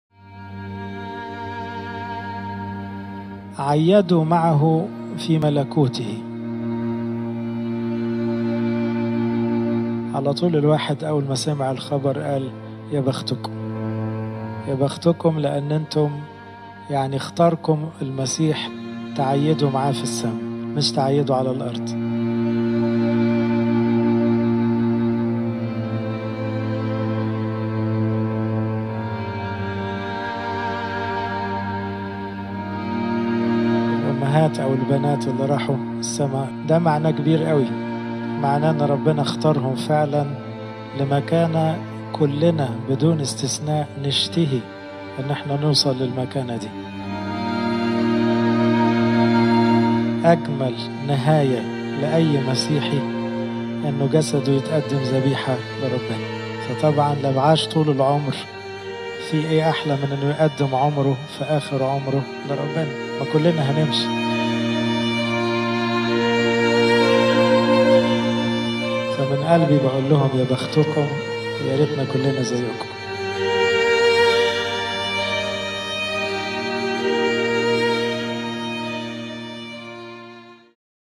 The Martyrs celebrated in His Heaven by Fr Daoud Lamei
“How lucky you are, because you're picked by the Lord Christ to celebrate the Feast with Him in Heaven”
Watch this short amazing words by Fr. Daoud Lamei